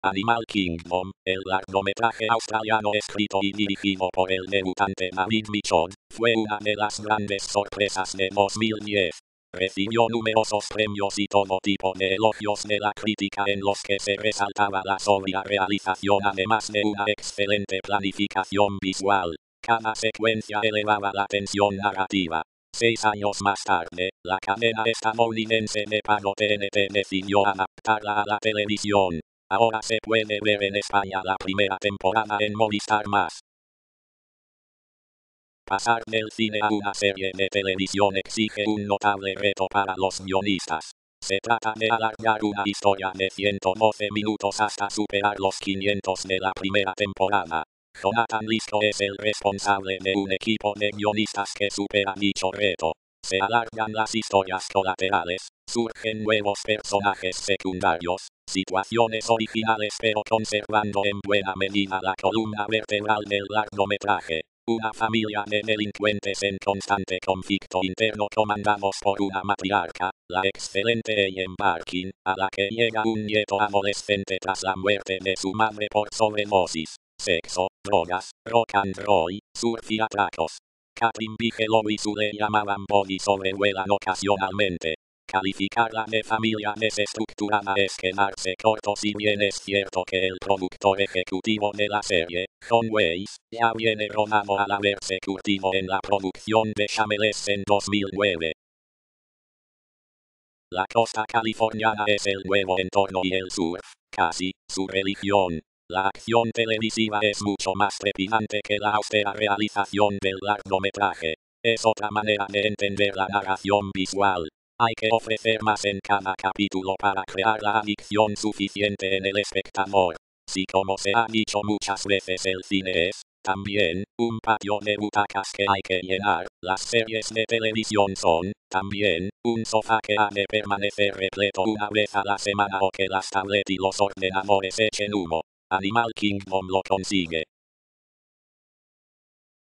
Echar humo
Animal Kingdom, el largometraje australiano escrito y dirigido por el debutante David Michôd, fue una de las grandes sorpresas de 2010. Recibió numerosos premios y todo tipo de elogios de la crítica en los que se resaltaba la sobria realización además de una excelente planificación visual: cada secuencia elevaba la tensión narrativa. Seis años más tarde, la cadena estadounidense de pago TNT decidió adaptarla a la televisión. Ahora se puede ver en España la primera temporada en Movistar +. V: ves_001378.mp4